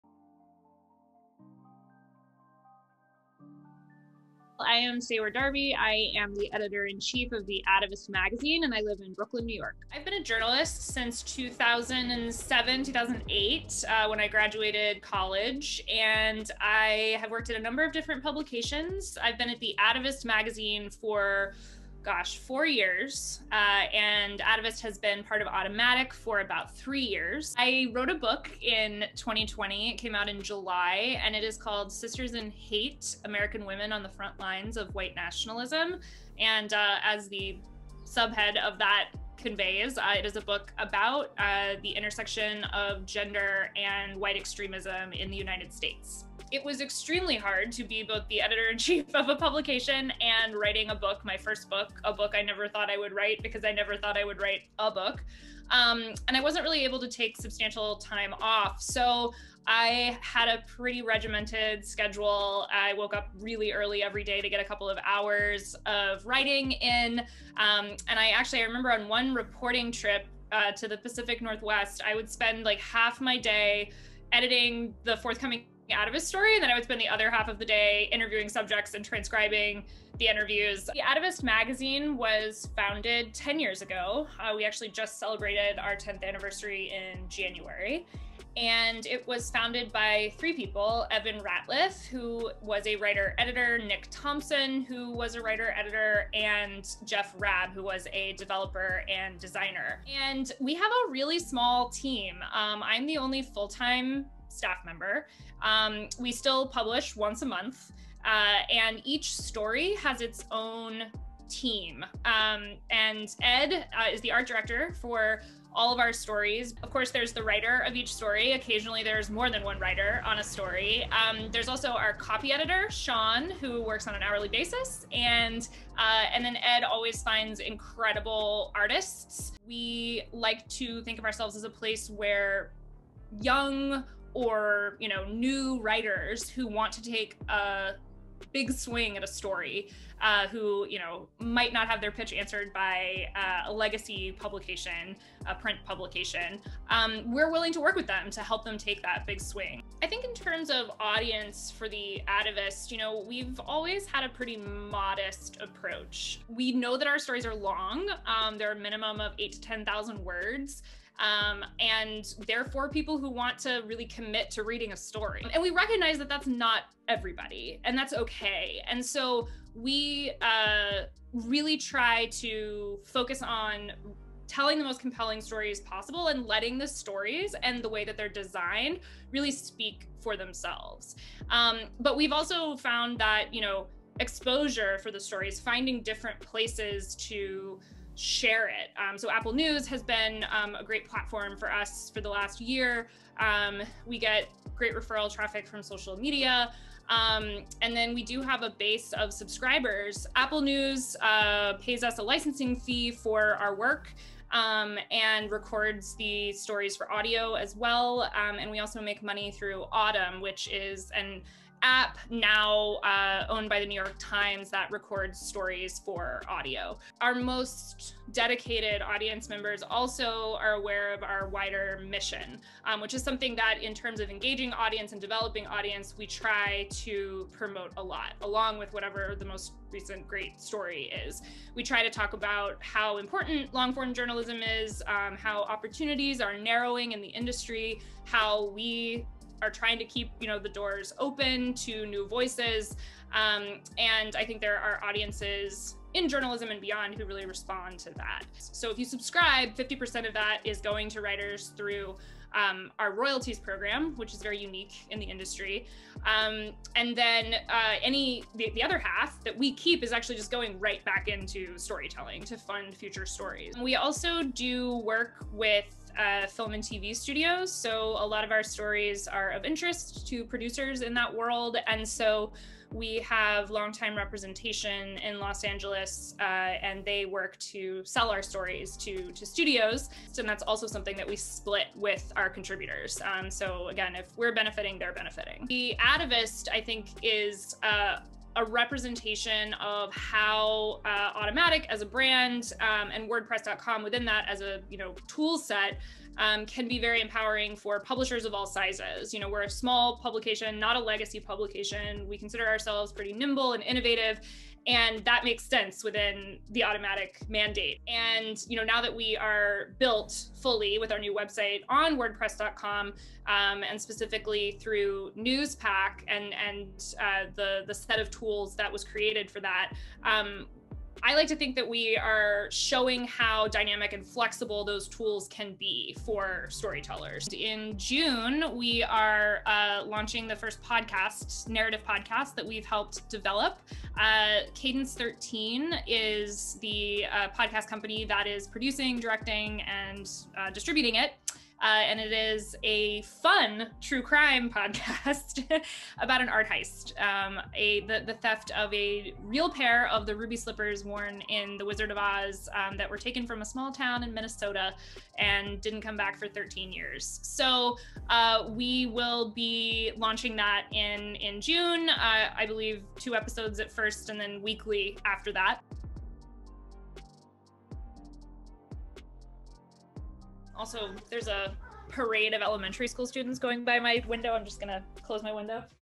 The Atavist and Seyward Darby | Grow with WordPress.com
In May 2021, we interviewed Seyward Darby, Editor in Chief of The Atavist.

The Atavist Magazine is known for its award-winning, in-depth journalism and creative, elegant design. Explore some of the web's best longform storytelling at magazine.atavist.com.